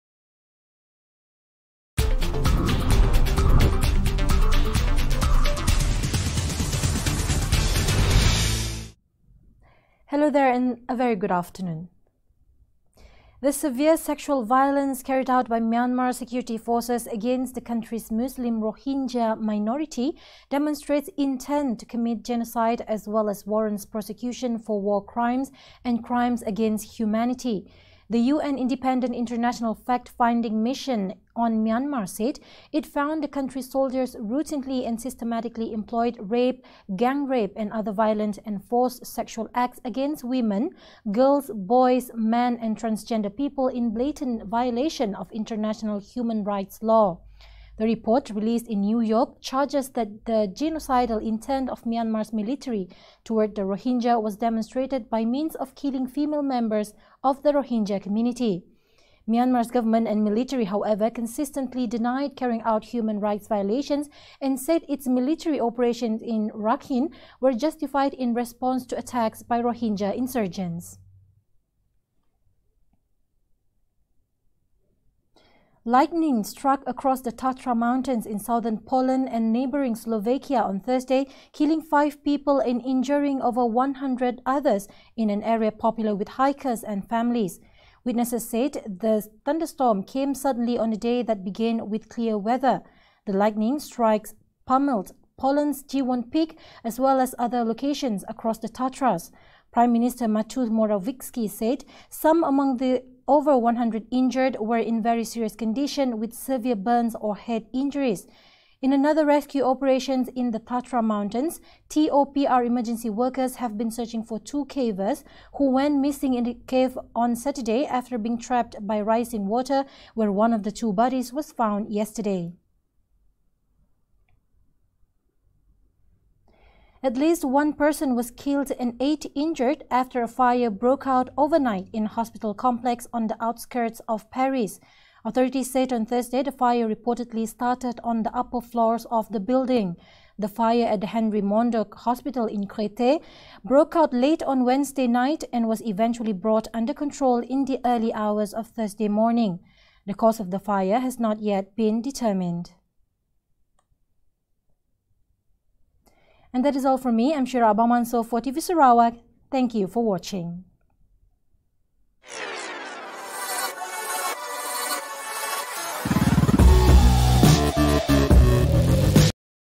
UN Report Condemns Sexual Violence By Myanmar Military
1. UN Report Condemns Sexual Violence By Myanmar Military

2. Lightning Strikes Kill 5, Injure Over 100 In Tatra Mountains

3. One Killed, 8 Injured After Fire Breaks Out In Paris Hospital